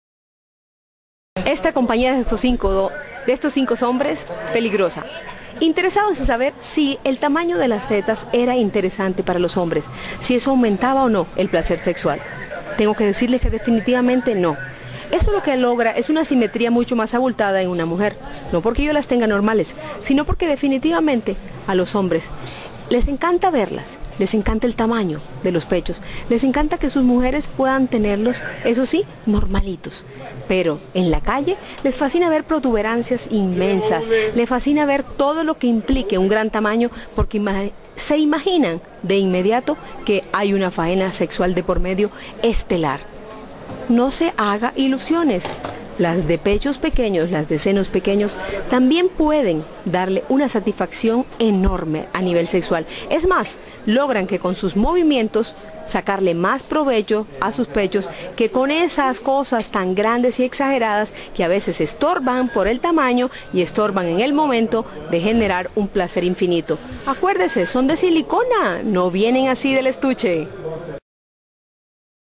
Videoblog: El tamaño de los senos
La doctora Carmen Larrazábal nos explica qué tanto importan el tamaño de los senos.